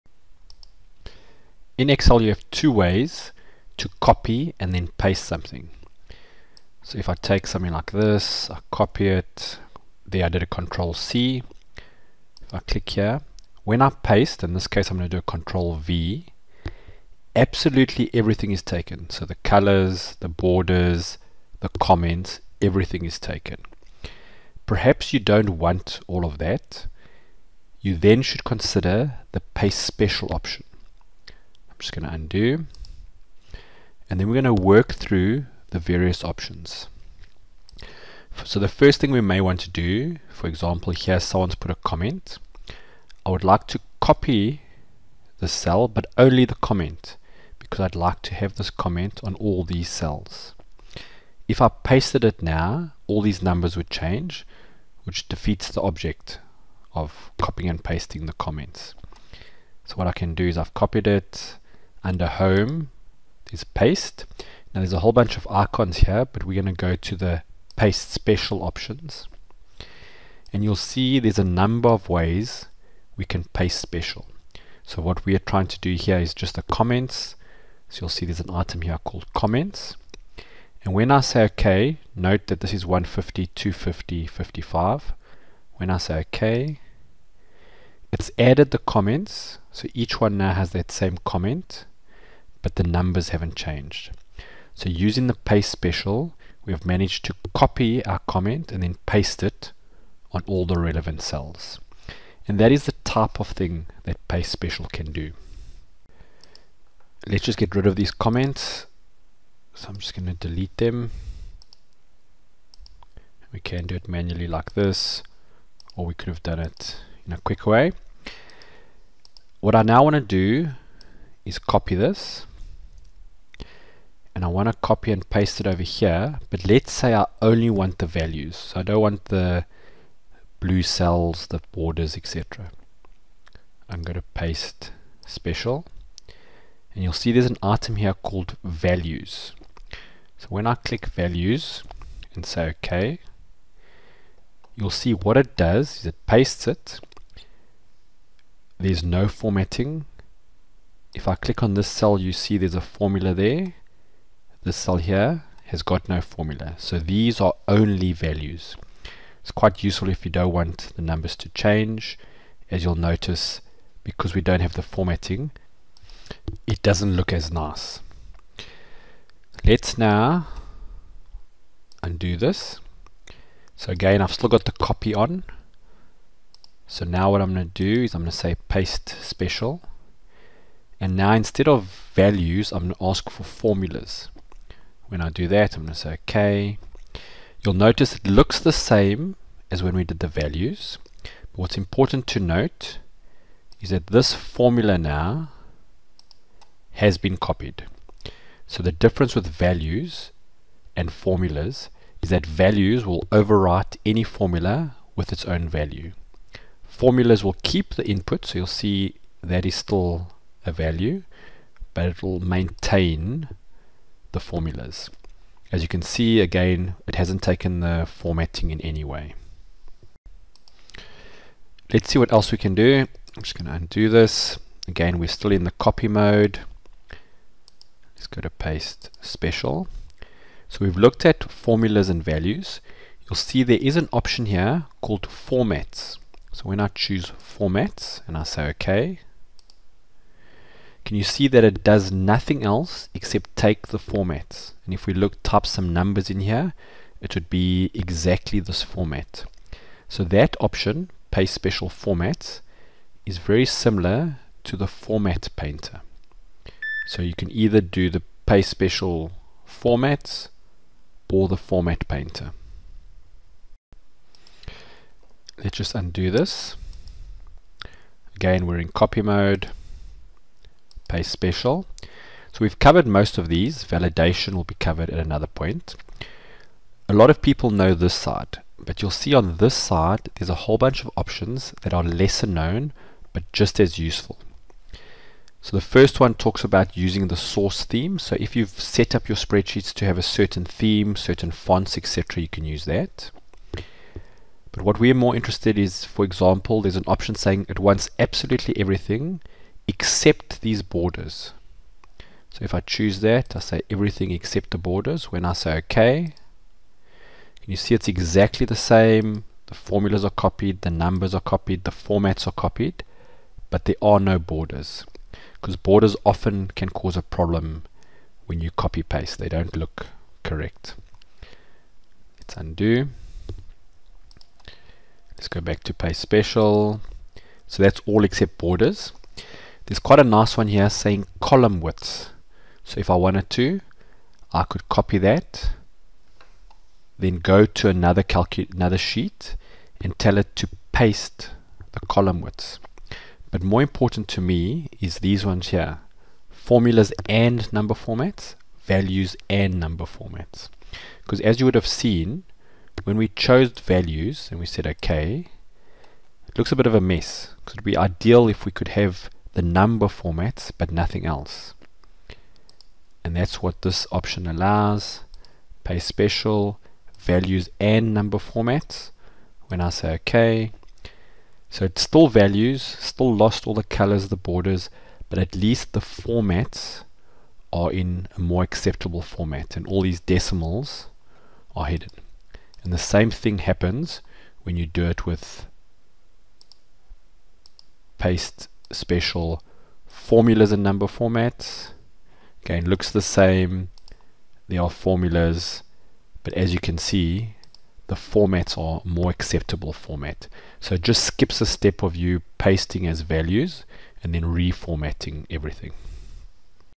Paste ONLY values OR comments OR values & format OR ... in Excel
00:00 Copy and Paste Special tool
00:10 Copy shortcut (CTRL + C) & Paste all shortcut (CTRL+ V)
00:40 How to copy and paste comments ONLY in Excel
02:00 Copy and paste ONLY the values (no borders, colors, formulas)
02:54 Paste without formatting (but keep numbers AND formulas)
03:44 Copy Paste formatting in Excel (without the format painter)
04:42 Copy Paste using the original Source Theme
04:57 Copy Paste WITHOUT the borders (useful with a Transpose)
05:23 Make all columns the same width in Excel
05:46 Excel paste values BUT keep number formatting
06:23 Paste numbers and formulas with number format but no colors and borders

The various uses of the Copy/ Paste Special tool including how to paste everything, only the numbers, numbers and formulas only (either with or without the number formatting), excluding borders and more.